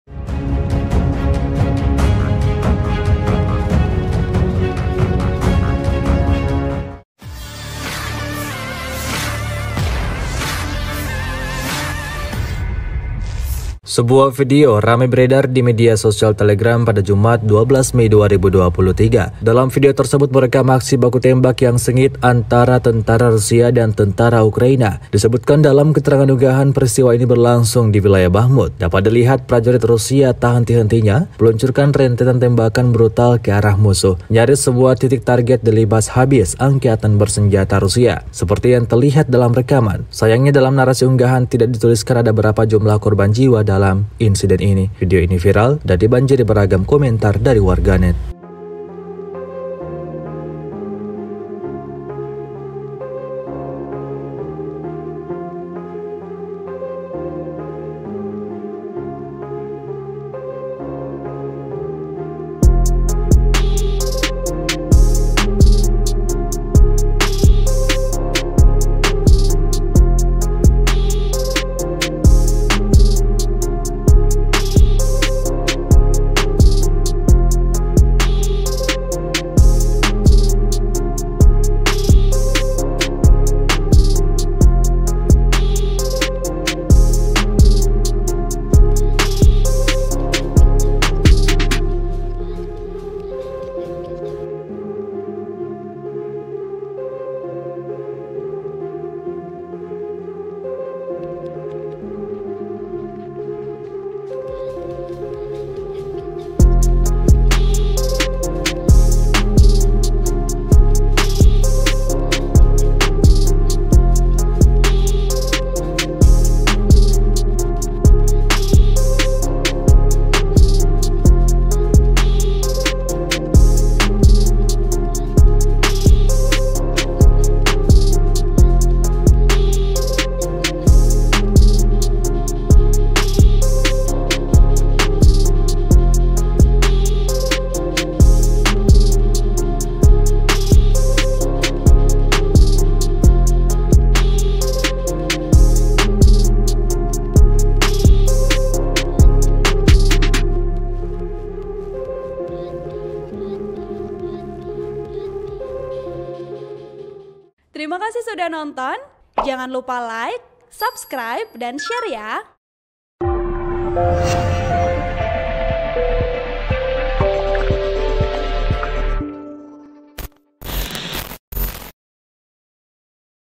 Tembak Brutal dari Jarak Dekat, Militer Rusia Disebut-sebut Tumbangkan Sejumlah Militan Ukraina
Dalam video tersebut merekam aksi baku tembak yang sengit antara tentara Rusia dan tentara Ukraina.

Disebutkan dalam keterangan unggahan peristiwa ini berlangsung di wilayah Bakhmut.

Dapat dilihat prajurit Rusia tak henti-hentinya meluncurkan rentetan tembakan brutal ke arah musuh.(*)

Narator: Milka Adriani